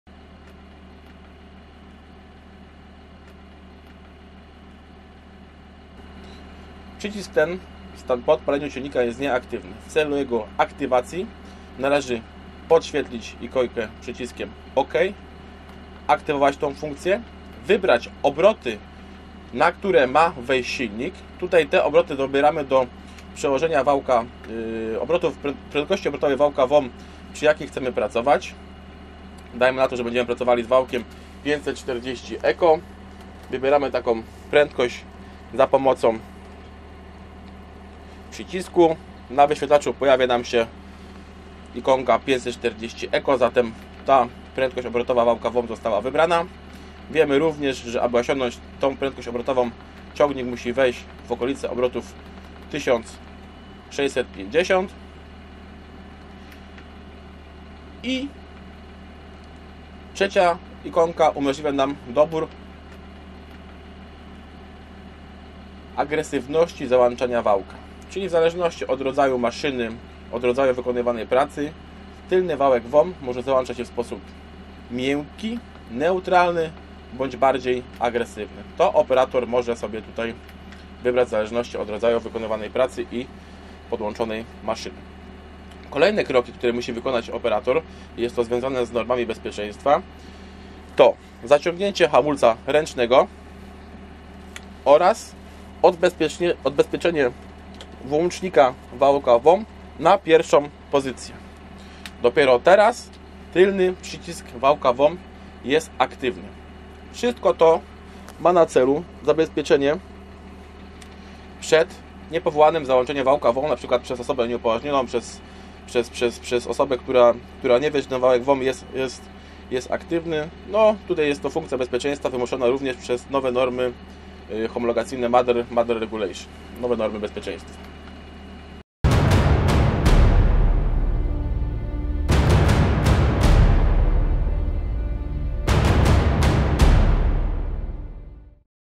Terminal SIS: Przycisk do włączania WOM - jak aktywować?
Dowiedz się jak aktywować wałek odbioru mocy (WOM) w ciągnikach MF serii S poprzez terminal SIS. Nasz doświadczony ekspert przedstawi to na prostym przykładzie, dzięki czemu dowiesz się w prosty i szybki sposób jak to zrobić. Polecamy sprawdzenie reszty równie merytorycznych filmów o terminalu SIS na naszej playliście.